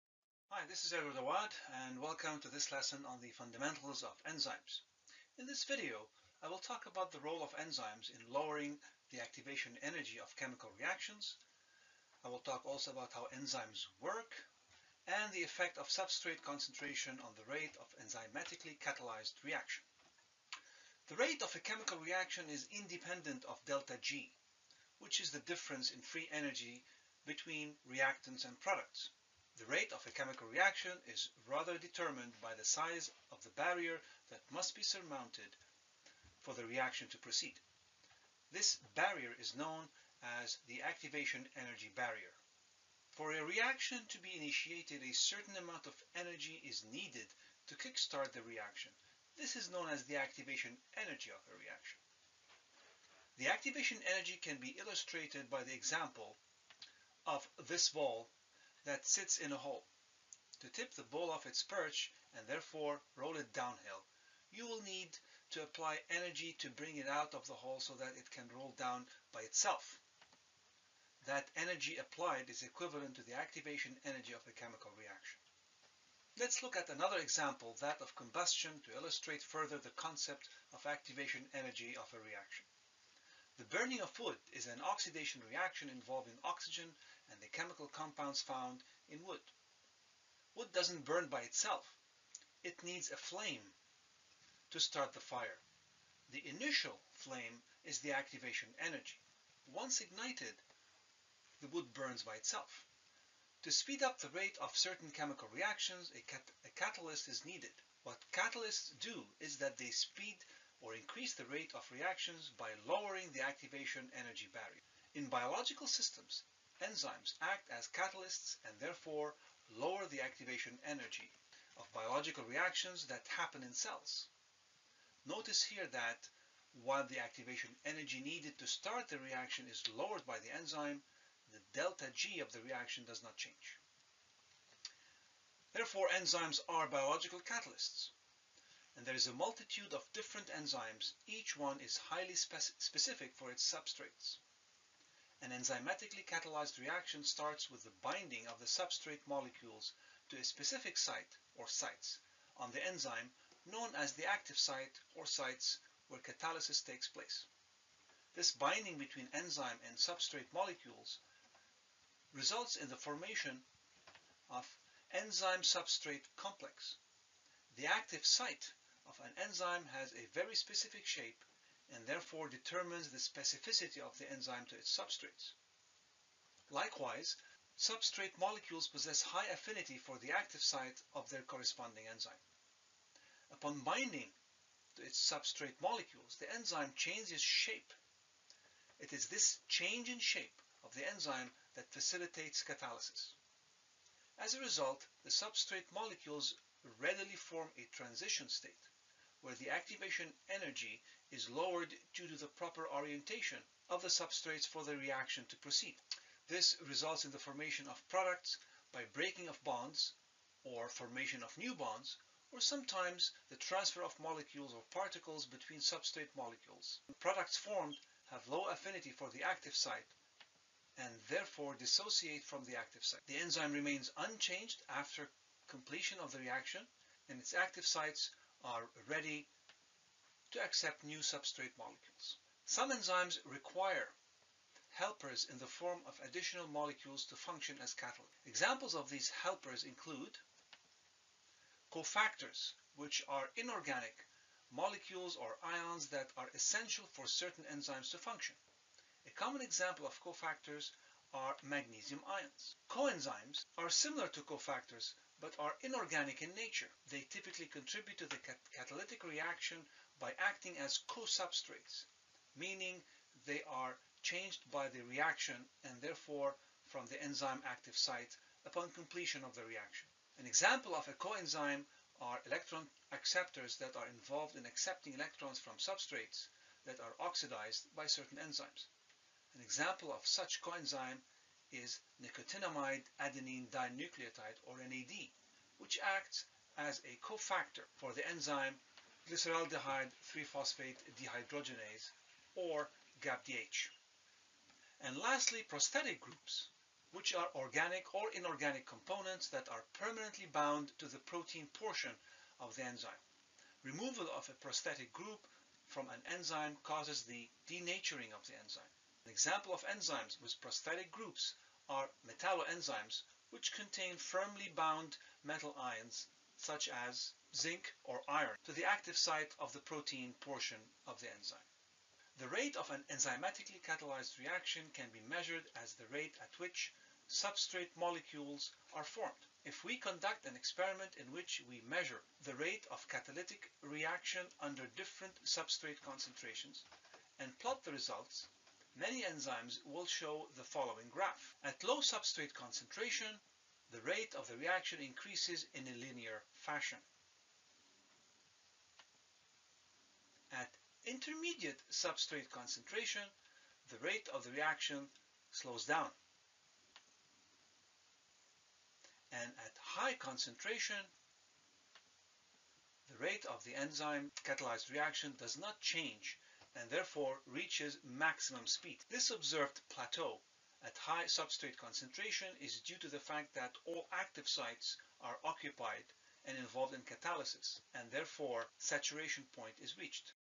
Enzymes
Table of Contents:

00:00 - Introduction
00:20 - Activation energy
01:43 - Biological catalysts
02:14 - How enzymes work
03:52 - Cofactors, coenzymes & prosthetic groups
05:22 - Substrate concentration & rate of enzymatic reaction